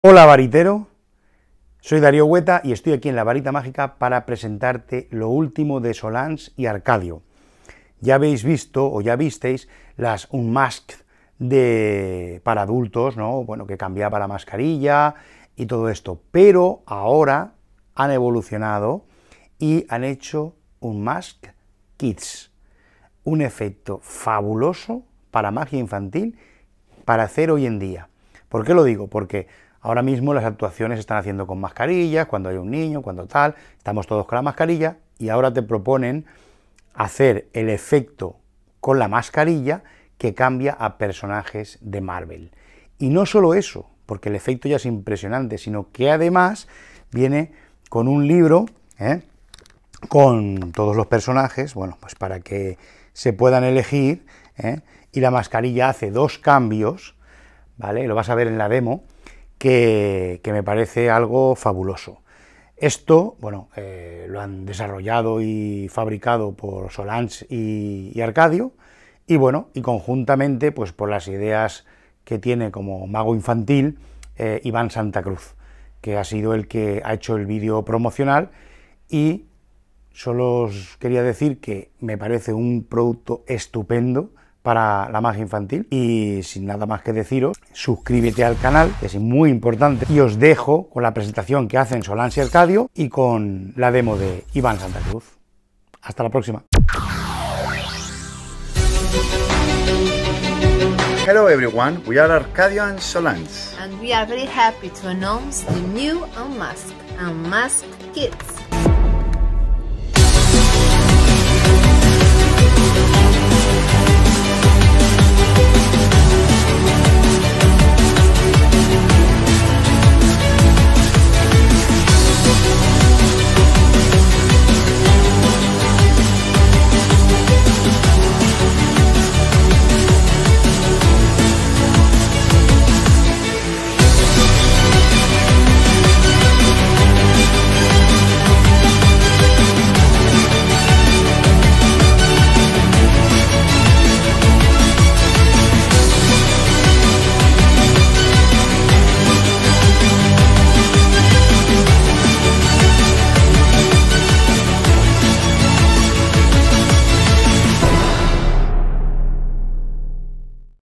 Unmasked Kids | Solange y Arkadio | Magia con mascarillas para magia Infanti l LaVarita.com ✔
Te presentamos un nuevo y original efecto para magia infantil
Después del gran éxito de ventas Unmasked, llega Unmasked Kids de la mano de Arkadio y Solange
Una auténtica revolución en la magia infantil ideal para la situación actual
Es el efecto perfecto tanto para magia infantil como familiar y se puede realizar en espectáculos de cerca, salón e incluso teatros
Además puedes llevar la máscara durante todo el show
Muy fácil de hacer.
La máscara ha sido testada Covid-19.
Sin ángulos.
El Reseteo es instantáneo.
Se puede presentar de distintas formas.
¿Que recibes?
Una máscara con 2 CAMBIOS.
Un libro de forzajes para 2 personajes (que te puede servir para otros usos)
Código QR con instrucciones de uso.
Este es un producto de alta calidad no te despistes y hazte con el tuyo.